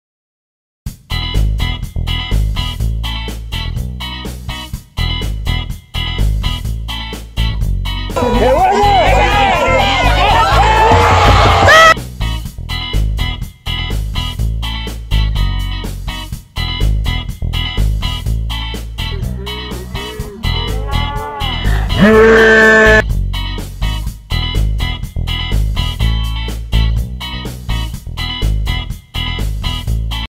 Anotaciones ganadoras en el GolCaterMotor de la semana
Llegó la hora del GolCaterMotor 💛🖤⚽

Una de estas anotaciones nos dio el paso a la semifinal de uno de los torneos más importantes de la capital FamiliaCaterMotor.